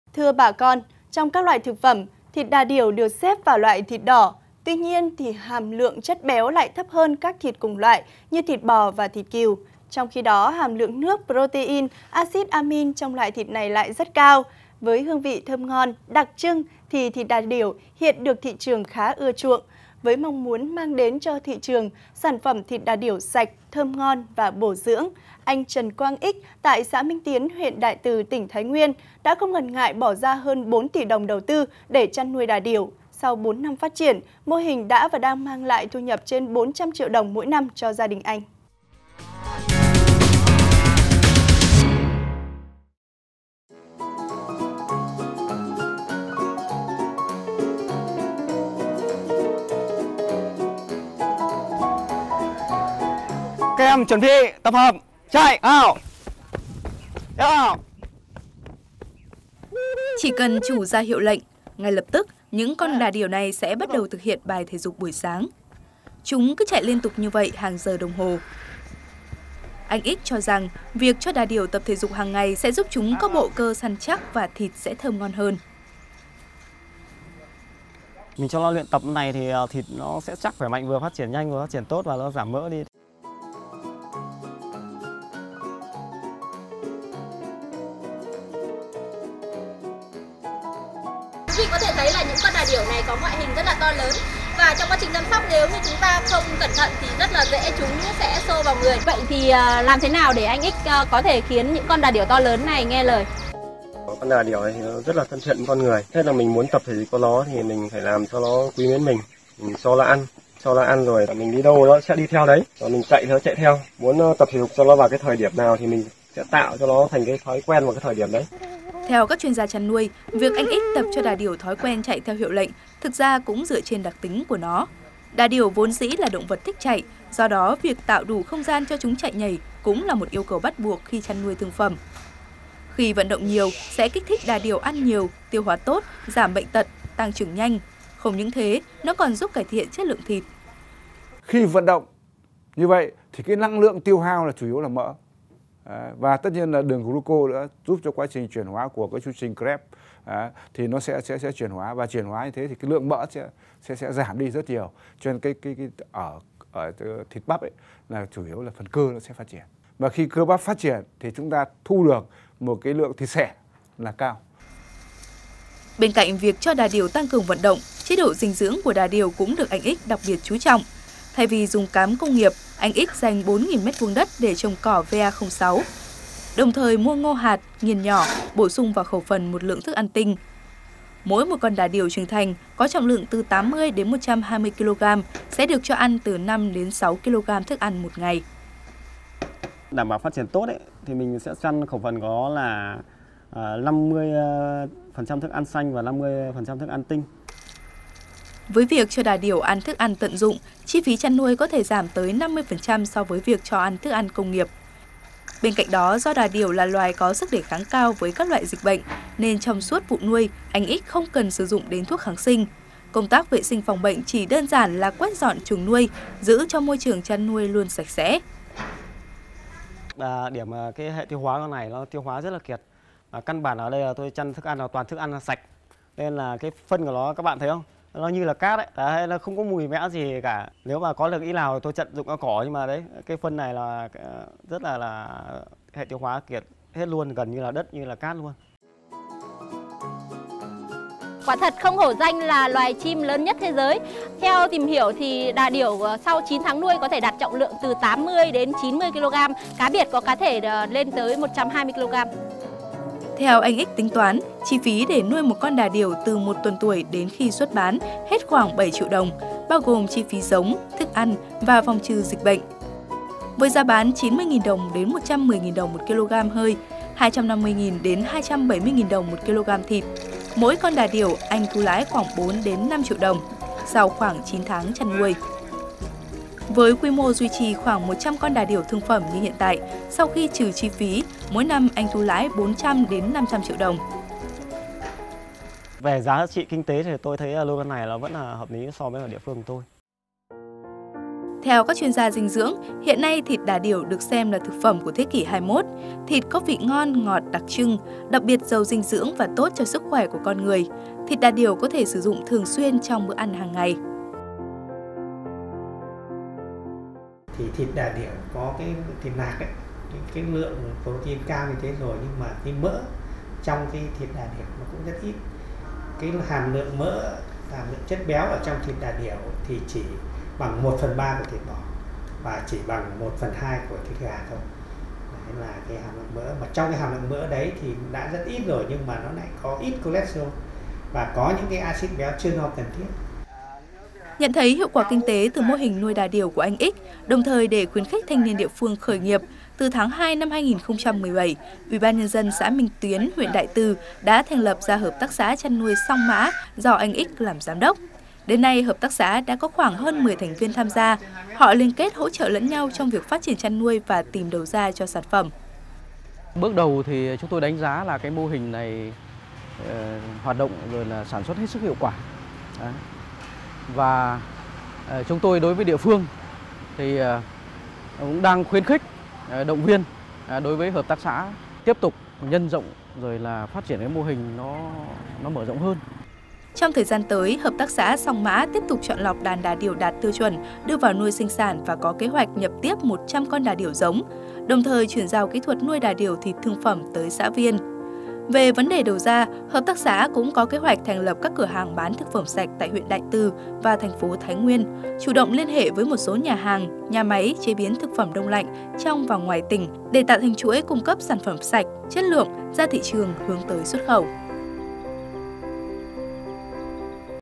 Nuôi đà điểu sạch, thu nửa tỷ mỗi năm I VTC16
VTC16 | Bỏ 4 tỷ đồng để nuôi đà điểu trên đất Đại Từ, Thái Nguyên, sau 4 năm, mô hình mang lại thu nhập gần nửa tỷ mỗi năm cho anh Trần Quang Ích.